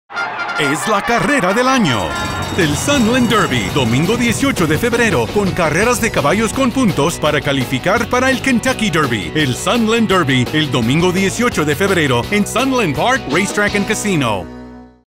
19th Running of the Sunland Derby
It's the race of the year!
The Sunland Derby you will get a chance to experience the Southwest’s most thrilling live racing action the sport has to offer. Witness historic races as some of the most elite thoroughbreds in world compete for a spot in history.

The 19th running of the Sunland Derby happens Sunday, February 18. Post Time: 12:25pm.